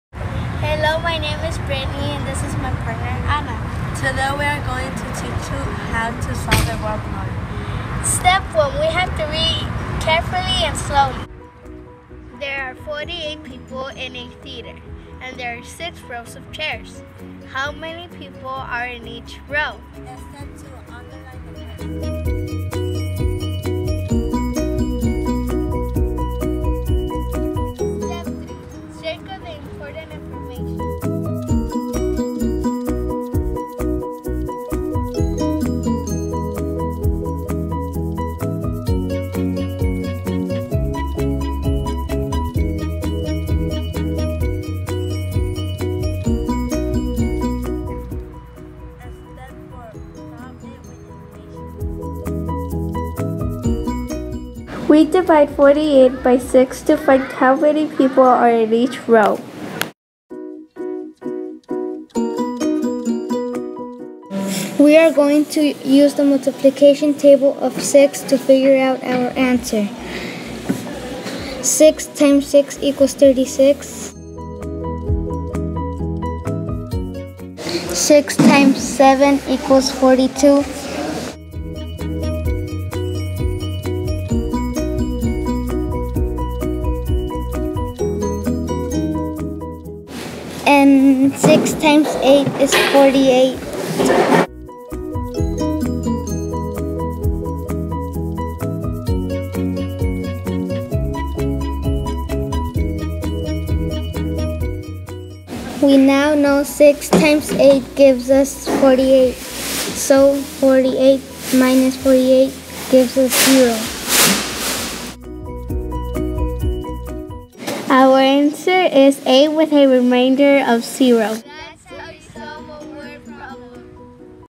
Word Problems
Students share a word problem and their technique to solve it.